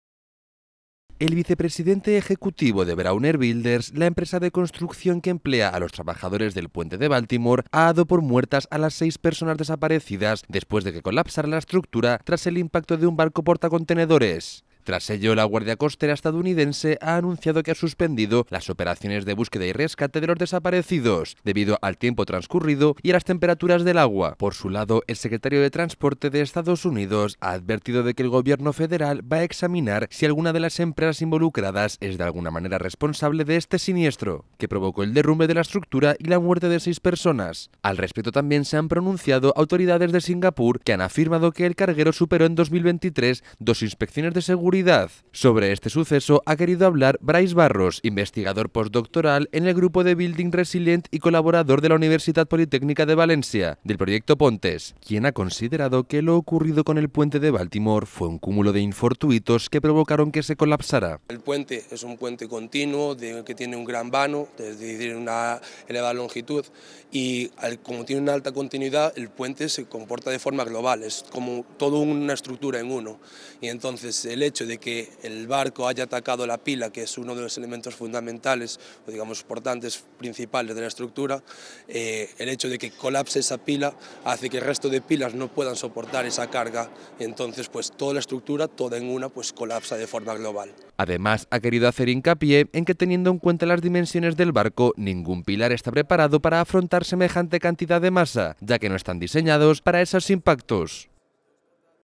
Brawner Builders da por muertos a sus seis trabajadores tras el colapso del puente de Baltimore
El vicepresidente ejecutivo de Brawner Builders, la empresa de construcción que emplea a los trabajadores del puente de Baltimore, ha dado por muertas a las seis personas desaparecidas después de que colapsara la estructura tras el impacto de un barco portacontenedores. Tras ello, la Guardia Costera estadounidense ha anunciado que ha suspendido las operaciones de búsqueda y rescate de los desaparecidos, debido al tiempo transcurrido desde que se derrumbó el puente y las temperaturas del agua.
Por su lado, el secretario de Transporte de Estados Unidos, Pete Buttigieg, ha advertido de que el Gobierno federal examinará si alguna de las empresas involucradas es de alguna manera responsable de este siniestro, que provocó el derrumbe de la estructura y la muerte de seis personas. Al respecto también han hablado las autoridades de Singapur, que han afirmado que el carguero superó en 2023 dos inspecciones de seguridad. Sobre este suceso, se ha pronunciado Brais Barros, investigador postdoctoral en el grupo de 'Building Resilient' y colaborador de la Universitat Politécnica de València del Proyecto Pontes, quien ha considerado que lo ocurrido con el puente de Baltimore fue un "cúmulo de fortuitos" que provocaron que se colapsara.
(Fuente: Europa Press / Baltimore)